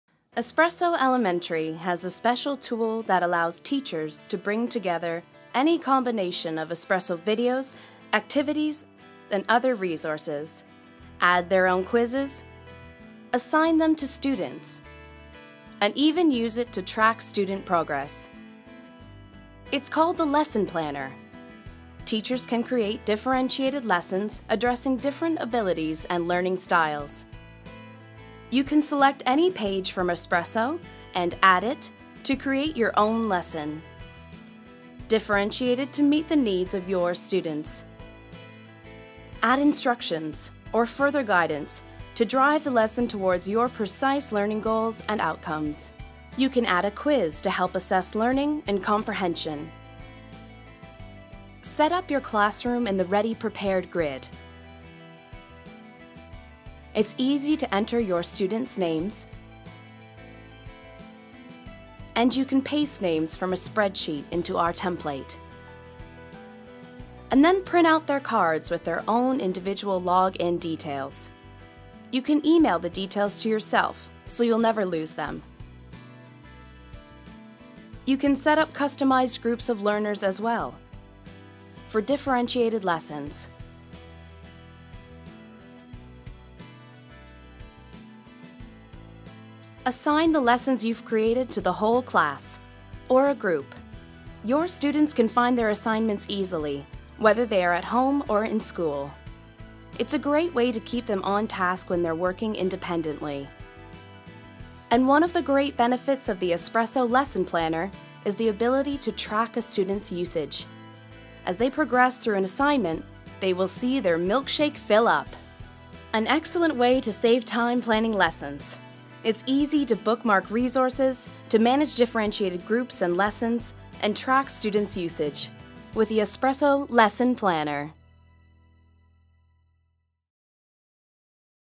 Create Differentiated Lessons
Create, assign and track student work with our Lesson Planner tool. This feature on Espresso Elementary allows you to combine resources, put in step-by-step order, add instruction or further guidance, create and add quizzes and assign to students. These lessons are ideal for teachers to create their own sequence of resources, for use in the classroom or for students to work through at their own pace at home.